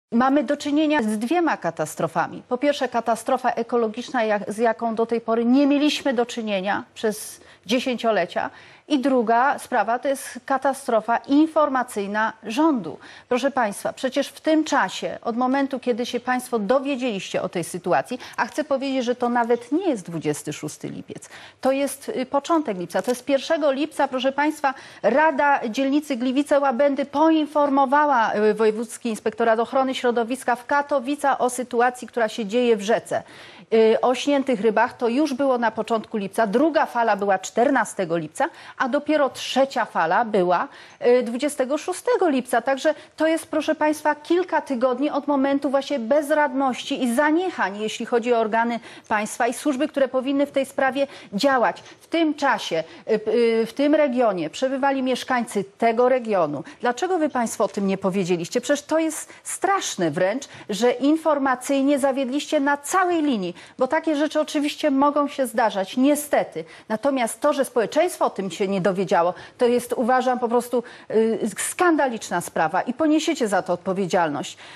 Karolina Pawliczak: Rząd ws. zatrutej Odry nie zrobił nic, bo był na urlopie | TVP INFO, 14.08.2022
Karolina Pawliczak: Indolencja, bezczynność i totalna amatorszczyzna! Ludzie od blisko miesiąca alarmowali o zagrożeniu, a rząd nie zrobił nic, bo był na urlopie. Zatruta, umierająca Odra to kolejna katastrofa, jaką zafundowali nam nieudacznicy z PiS. Który to już raz udowadniacie, że nie nadajecie się do rządzenia? Odejdźcie jak najszybciej i przestańcie szkodzić Polsce!